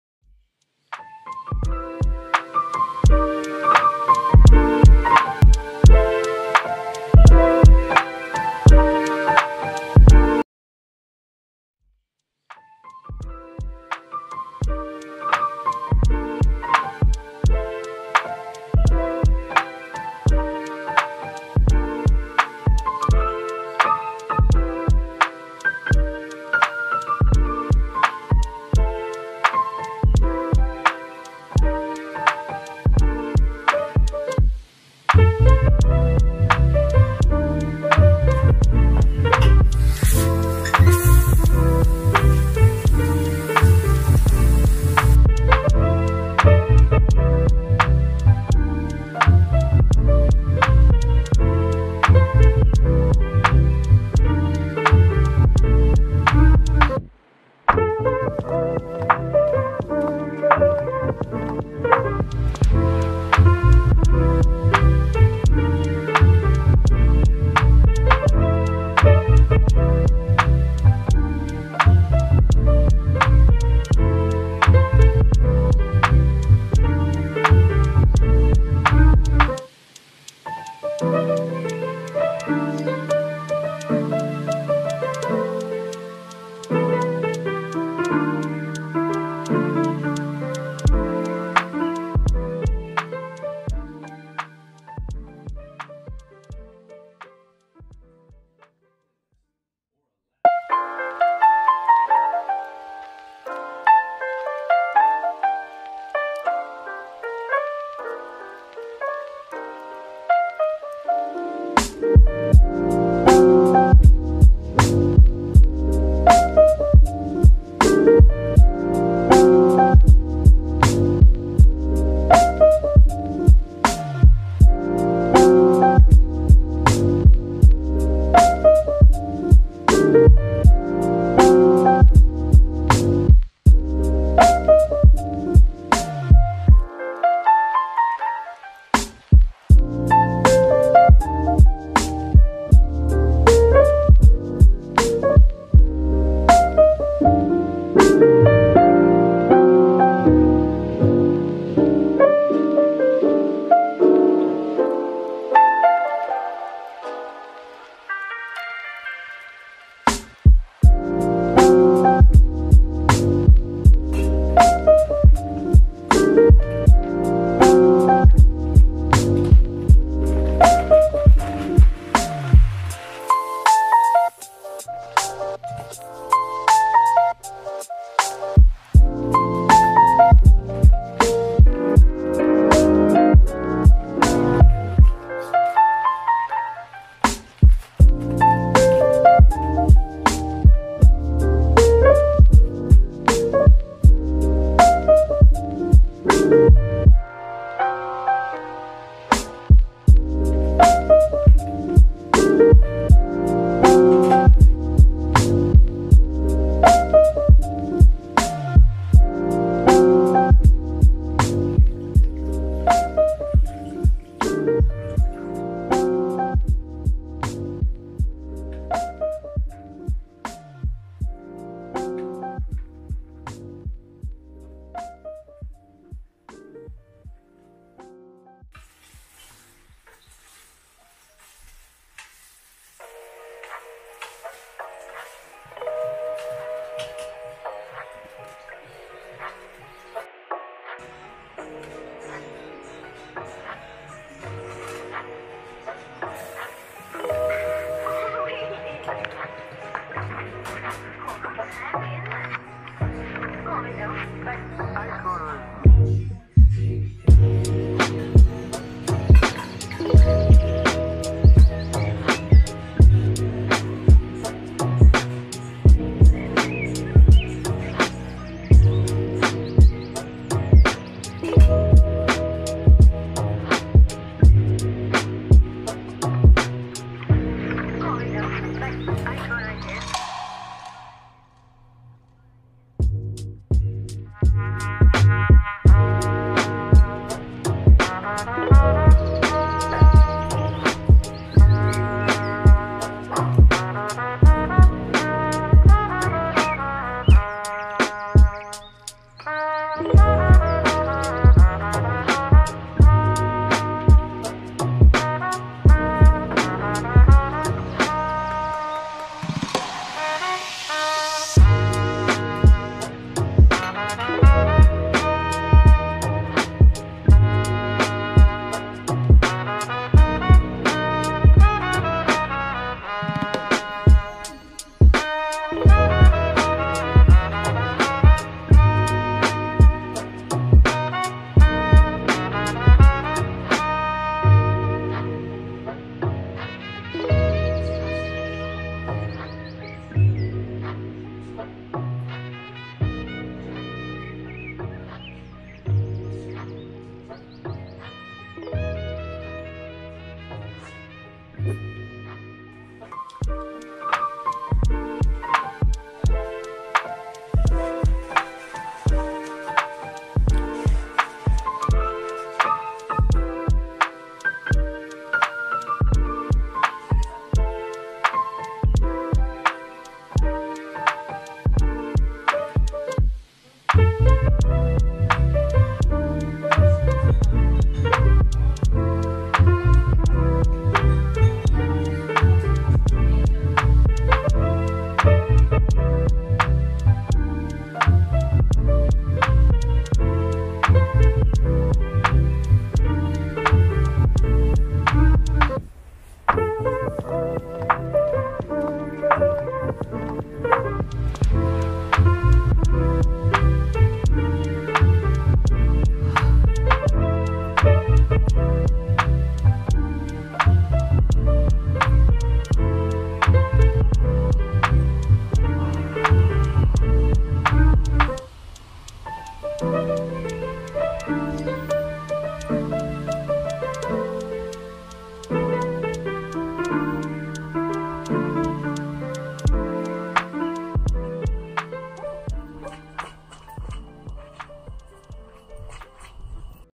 H O M E V L O G // Korean Drama, Cooking Filippino food, Snow!
Just Trying my best to do more at home by cooking and loving my pet Francis :)